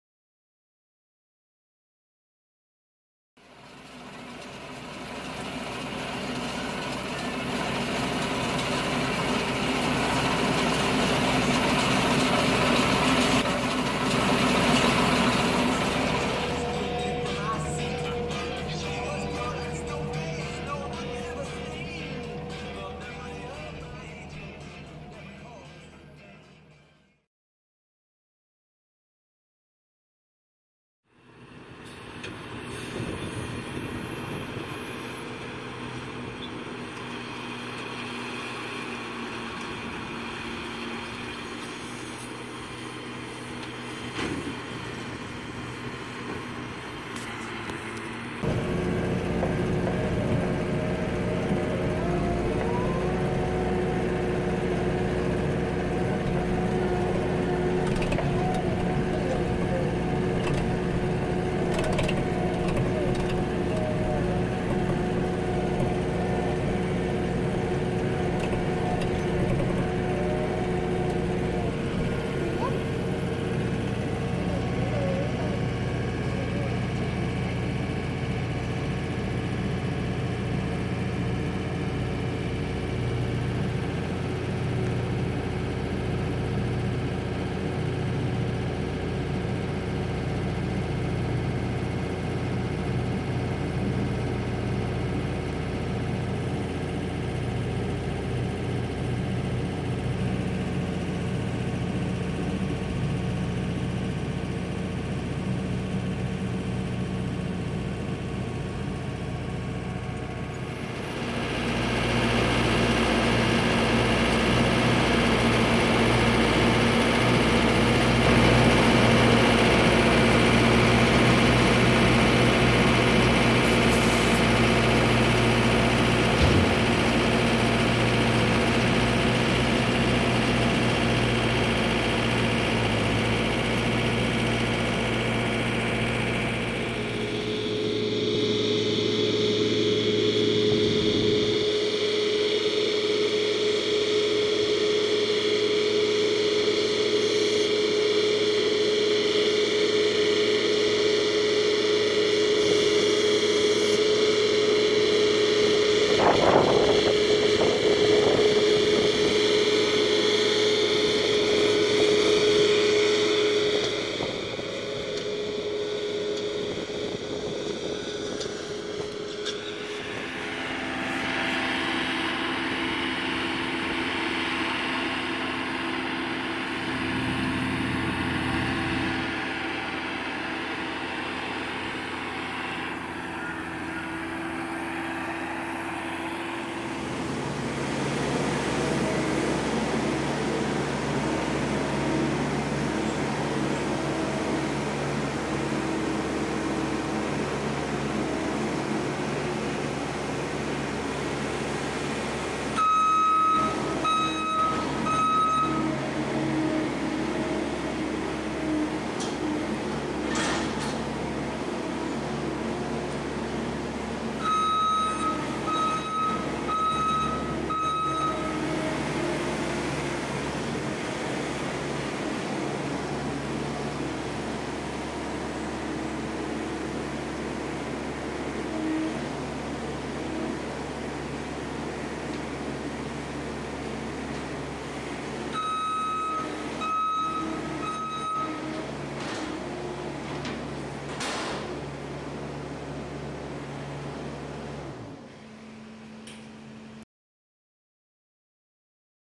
Making Hay John Deere Tractors 2017
2017 Hay baling is upon us, the John Deere Tractors and balers are warmed up and hit the fields in force out here baling hay.